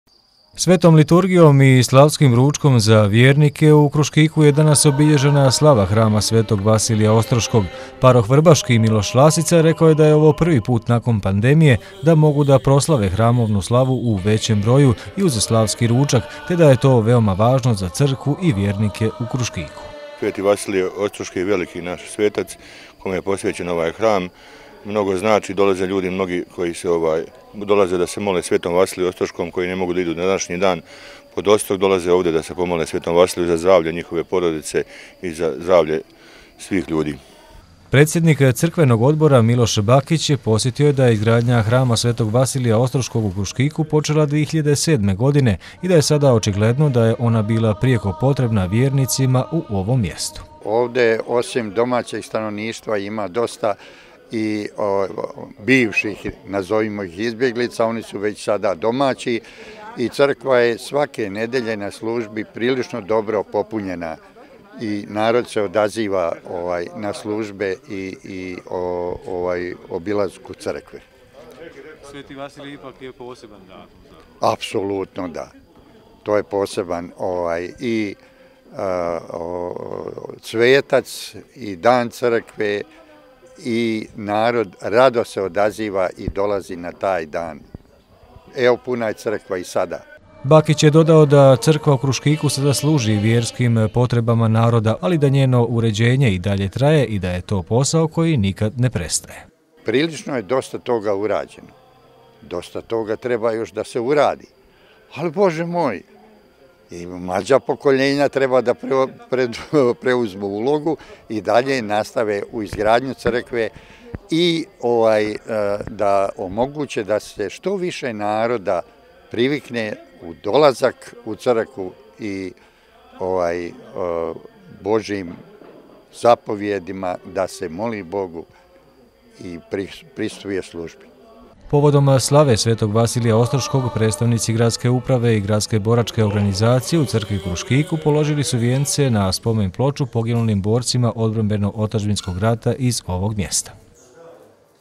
Obilježena slava Hrama Svetog Vasilija Ostroškog u Kruškiku
Hram Svetog Vasilija Ostroškog u naselju Kruškik u Gradišci obilježio je danas krsnu slavu. Paroh vrbaški Miloš Lasica rekao je da veliki broj vjernika sa područja Gradiške i okoline koji nisu u prilici da danas posjete Ostrog dolaze u crkvu u Kruškiku da se pomole Svetom Vasiliju Čudotvorcu.